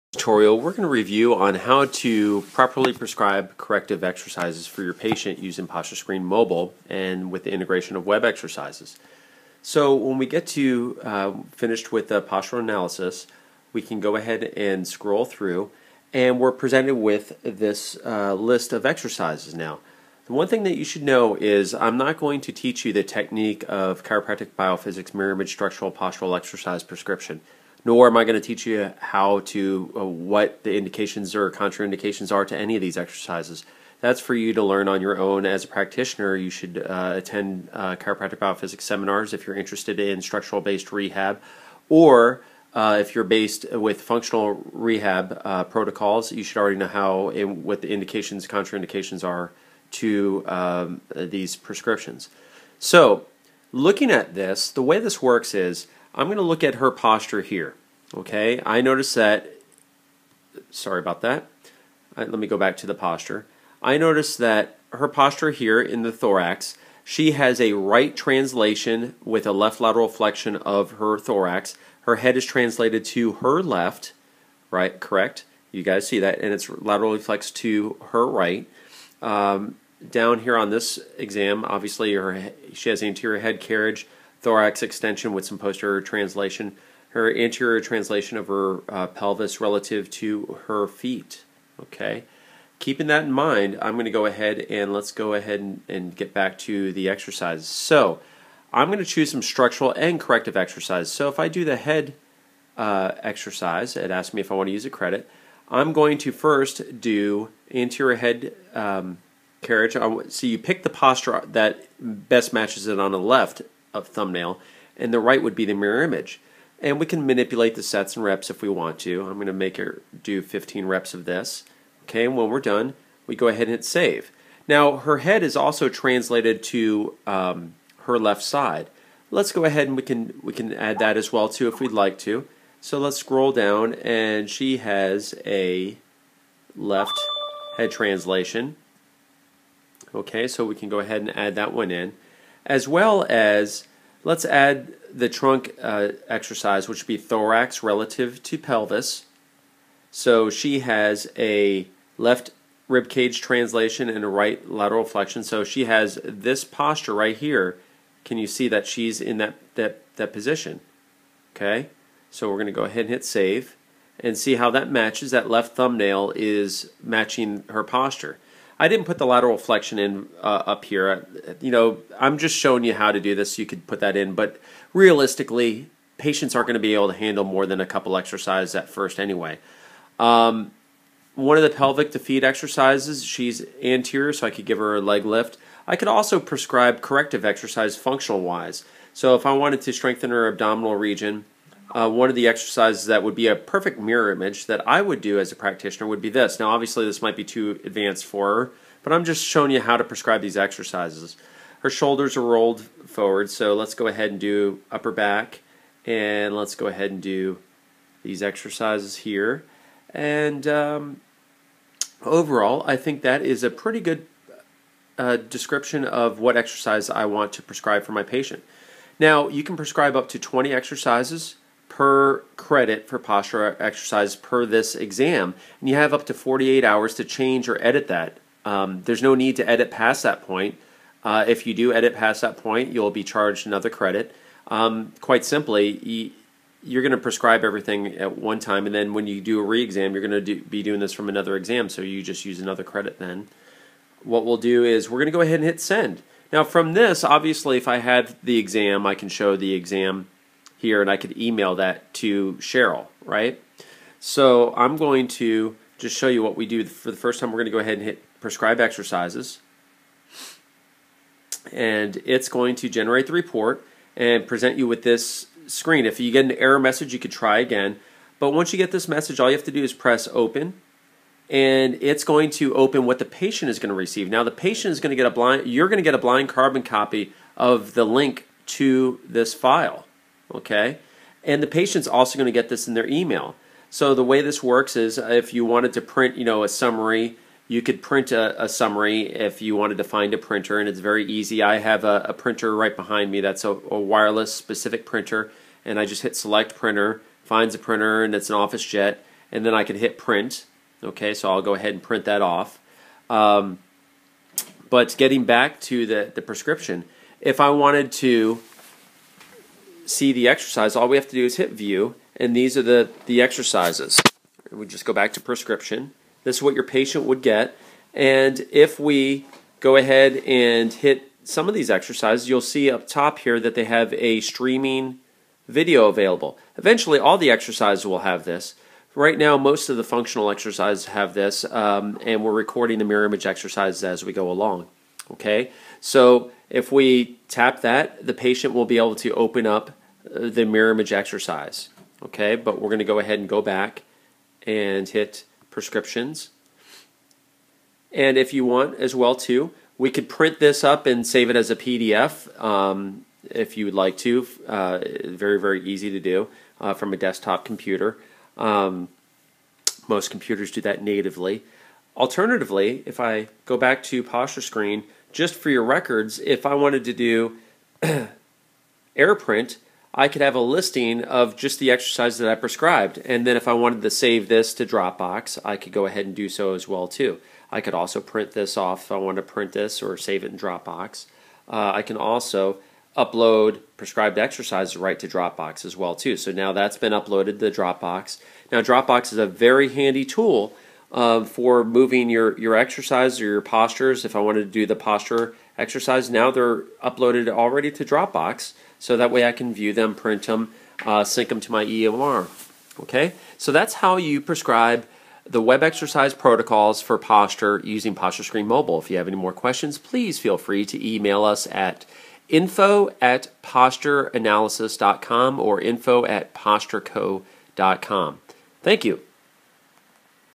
WebExercises on PostureScreen Mobile
This is a basic tutorial on how to use the new exercise module within the PostureScreen Mobile